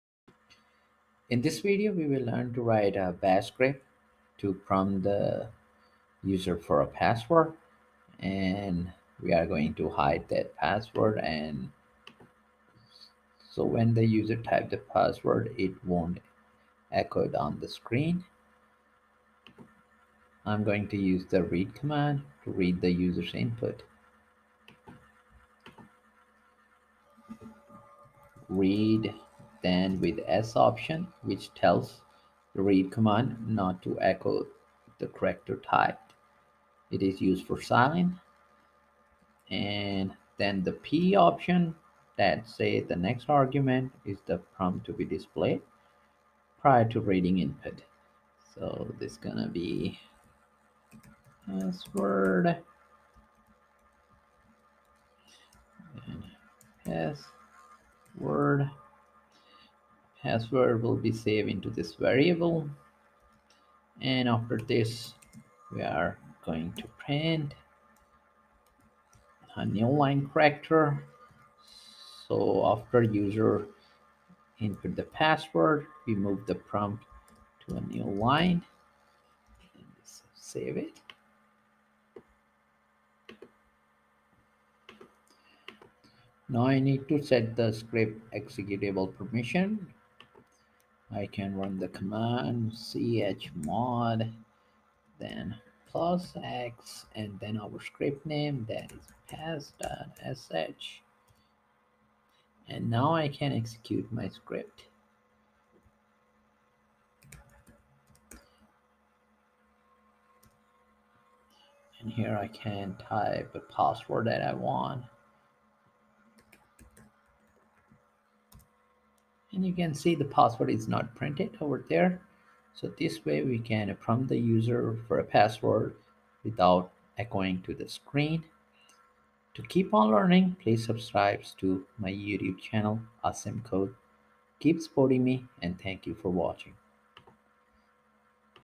Bash script to Prompt the user for a Password
In this video we will learn how to Prompt the user for a Password without echoing to terminal.